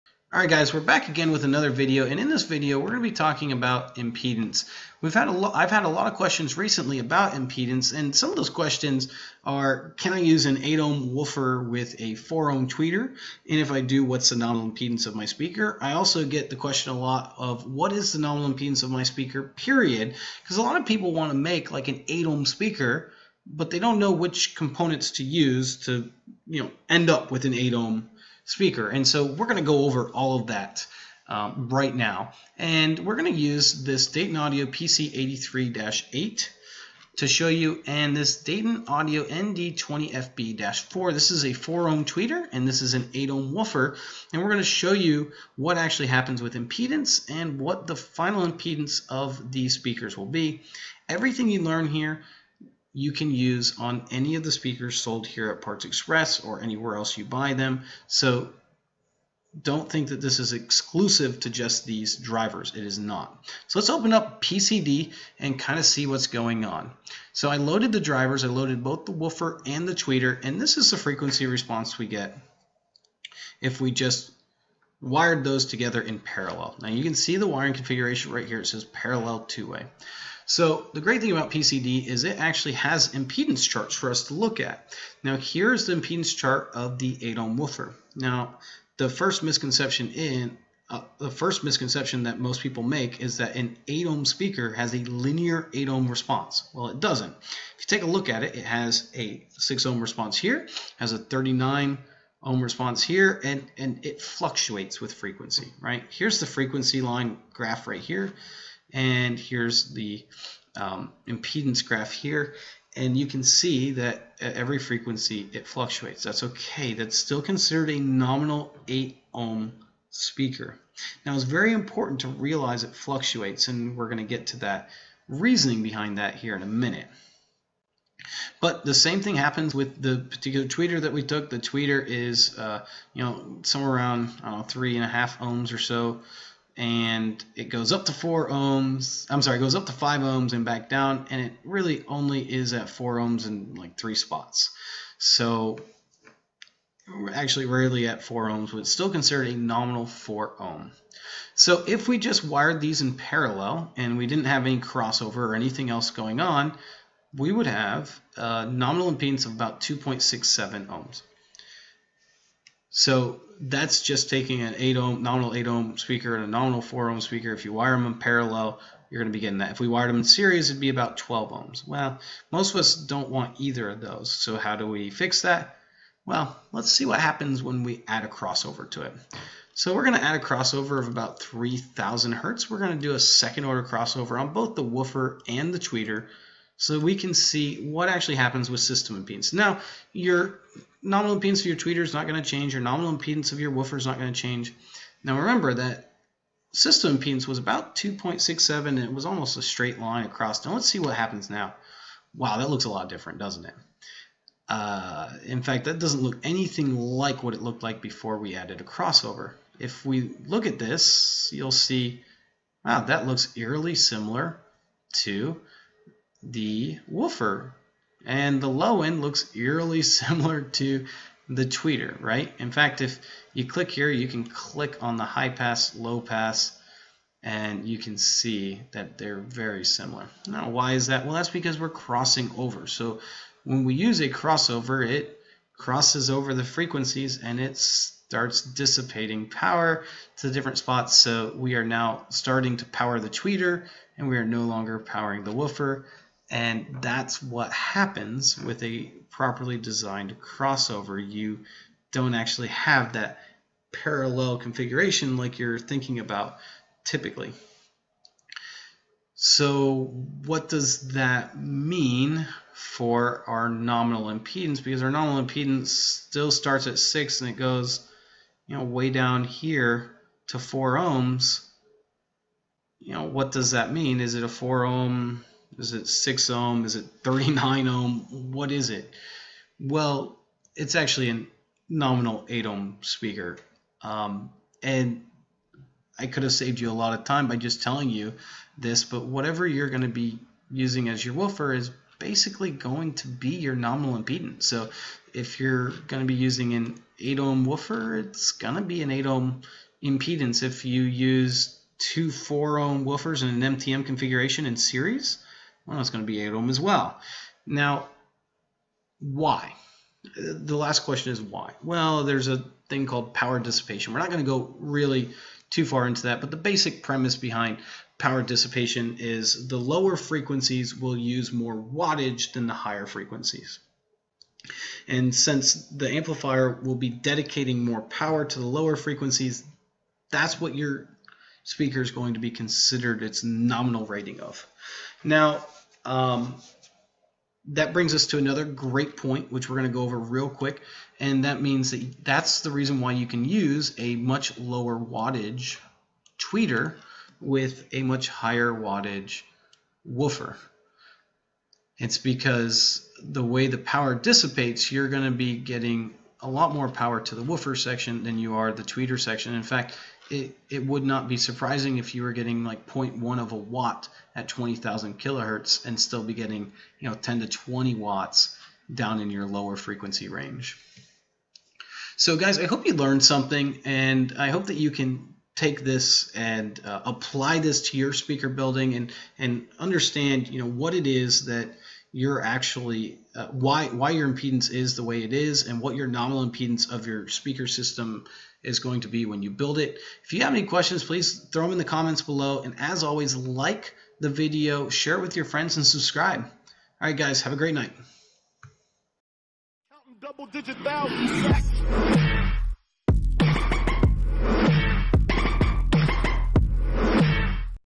Can I use an 8ohm woofer with 4 ohm tweeter? Impedance Questions Explained
Can you use an 8 ohm woofer with a 4 ohm tweeter?  Do they need to match?  What is the final impedance of my speaker build?  All of these questions are answered in this quick video.